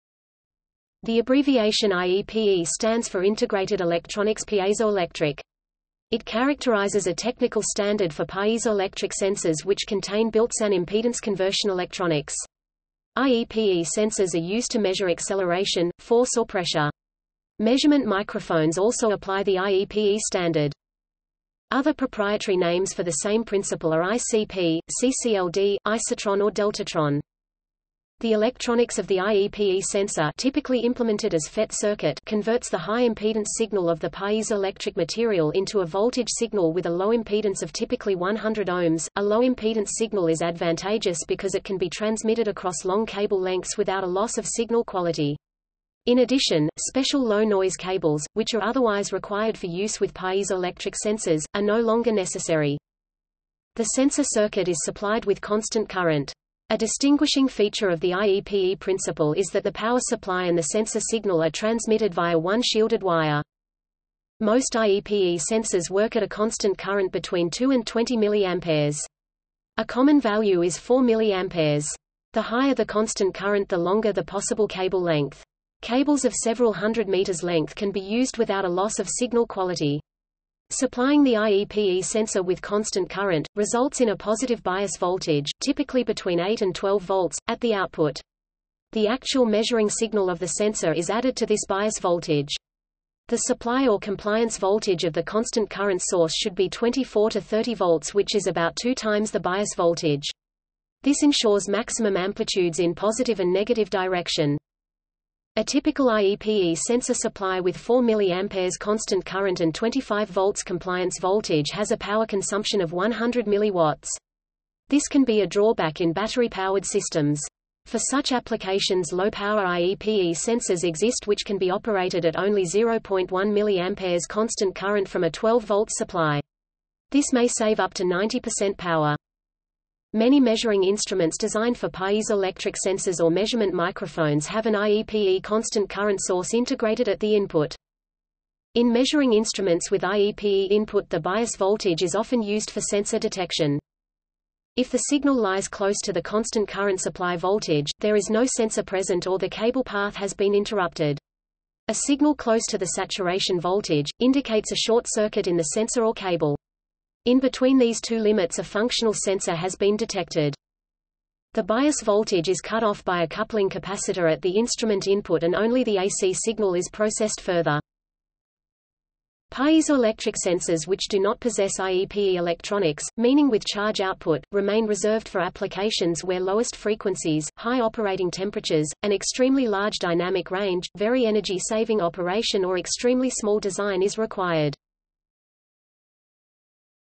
Integrated circuit piezoelectric sensor | Wikipedia audio article
- Socrates

SUMMARY
The abbreviation IEPE stands for Integrated Electronics Piezo-Electric. It characterises a technical standard for piezoelectric sensors which contain built-in impedance conversion electronics. IEPE sensors are used to measure acceleration, force or pressure. Measurement microphones also apply the IEPE standard.
Other proprietary names for the same principle are ICP, CCLD, IsoTron or DeltaTron.
The electronics of the IEPE sensor (typically implemented as FET circuit) converts the high impedance signal of the piezoelectric material into a voltage signal with a low impedance of typically 100 Ω. A low impedance signal is advantageous because it can be transmitted across long cable lengths without a loss of signal quality.  In addition, special low noise cables, which are otherwise required for use with piezoelectric sensors, are no longer necessary.
The sensor circuit is supplied with constant current. A distinguishing feature of the IEPE principle is that the power supply and the sensor signal are transmitted via one shielded wire.
Most IEPE sensors work at a constant current between 2 and 20 mA. A common value is 4 mA. The higher the constant current the longer the possible cable length. Cables of several hundred meters length can be used without a loss of signal quality. Supplying the IEPE sensor with constant current, results in a positive bias voltage, typically between 8 and 12 volts, at the output. The actual measuring signal of the sensor is added to this bias voltage. The supply or compliance voltage of the constant current source should be 24 to 30 V which is about two times the bias voltage. This ensures maximum amplitudes in positive and negative direction.
A typical IEPE sensor supply with 4 mA constant current and 25 V compliance voltage has a power consumption of 100 mW. This can be a drawback in battery powered systems. For such applications low-power IEPE sensors exist which can be operated at only 0.1 mA constant current from a 12 V supply. This may save up to 90 % power.

Many measuring instruments designed for piezoelectric sensors or measurement microphones have an IEPE constant current source integrated at the input.
In measuring instruments with IEPE input the bias voltage is often used for sensor detection.
If the signal lies close to the constant current supply voltage, there is no sensor present or the cable path has been interrupted. A signal close to the saturation voltage, indicates a short-circuit in the sensor or cable. In between these two limits a functional sensor has been detected.
The bias voltage is cut off by a coupling capacitor at the instrument input and only the AC signal is processed further.

Piezoelectric sensors which do not possess IEPE electronics, meaning with charge output, remain reserved for applications where lowest frequencies, high operating temperatures, an extremely large dynamic range, very energy saving operation or extremely small design is required.